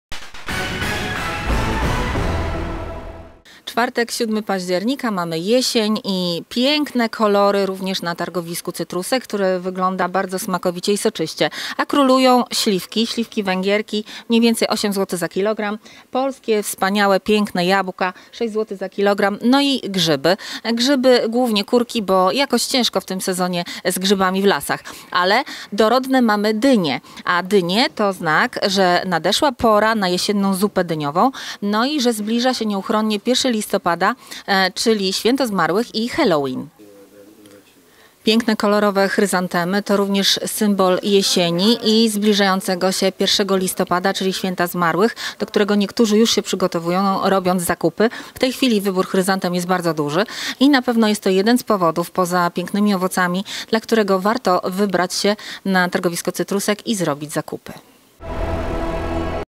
Dzień dobry, tu Opole
Dynie, grzyby, śliwki, jabłka - to symbole jesieni. Rozpoczął się też sezon na chryzantemy.